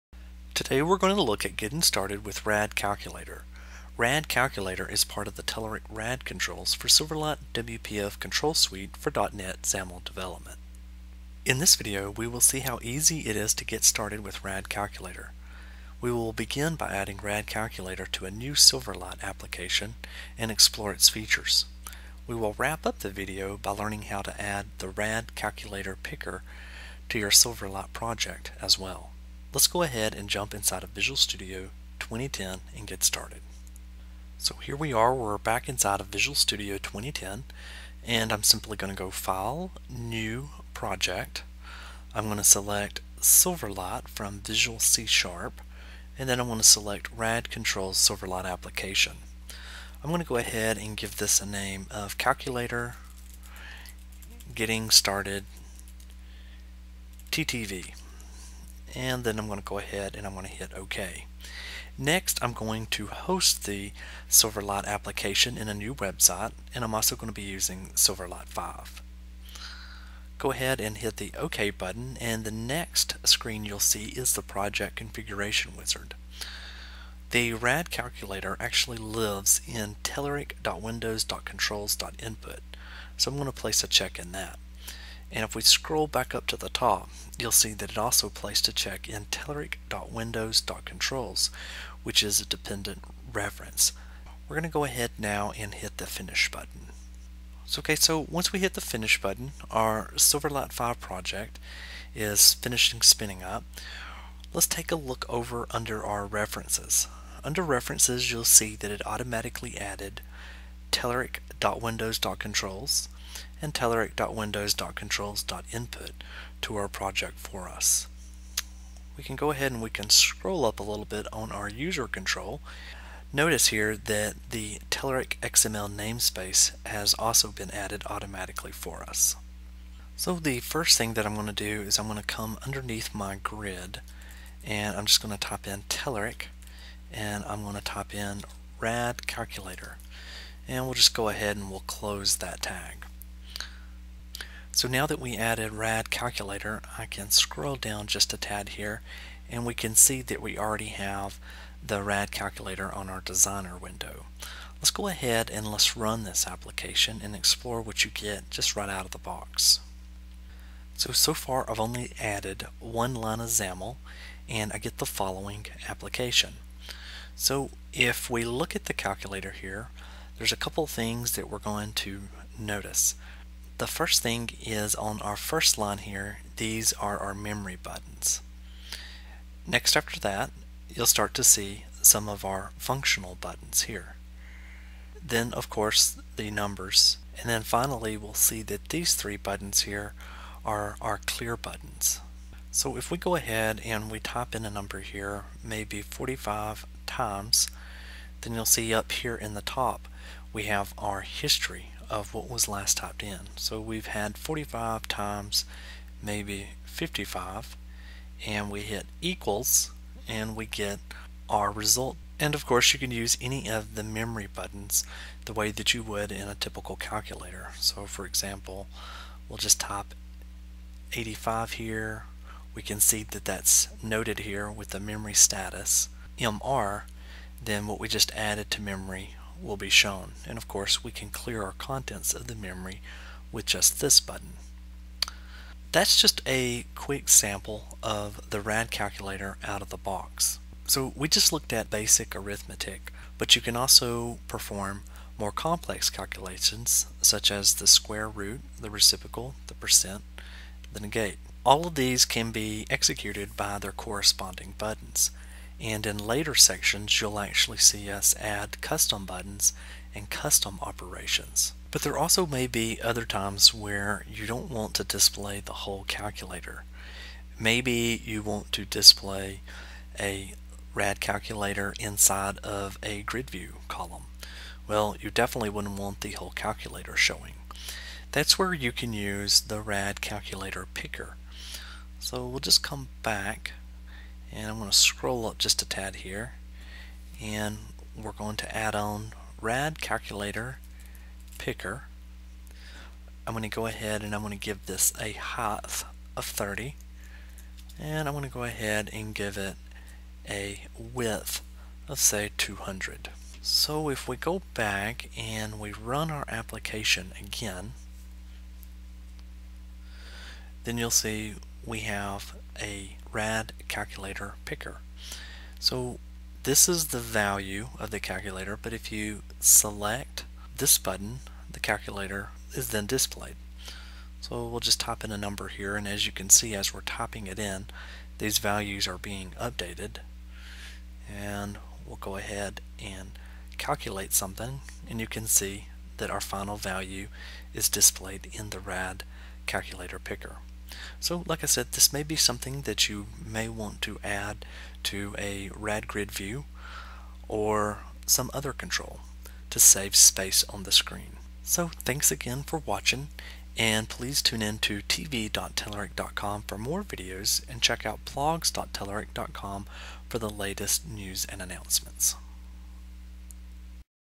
Calculator - Part 1: Getting Started (Silverlight & WPF)
In this video, we will see how easy it is to get started with RadCalculator. We will begin by adding RadCalculator to a new Silverlight Application and explore its features. We will wrap up the video by learning how to add the RadCalculatorPicker to you Silverlight project as well.